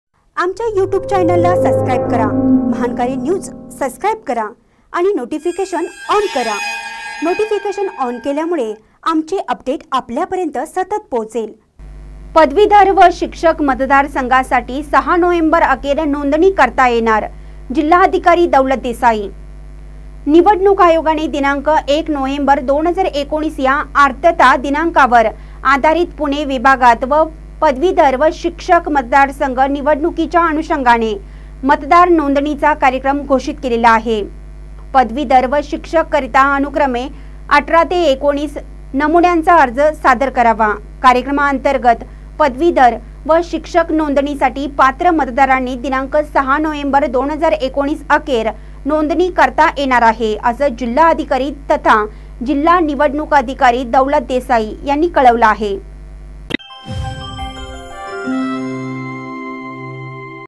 पदवीधर व शिक्षक मतदार संघासाठी ६ नोव्हेंबर अखेर नोंदणी करता येणार -
पदवीधर व शिक्षक मतदार संघासाठी ६ नोव्हेंबर अखेर नोंदणी करता येणार - जिल्हाधिकारी दौलत देसाई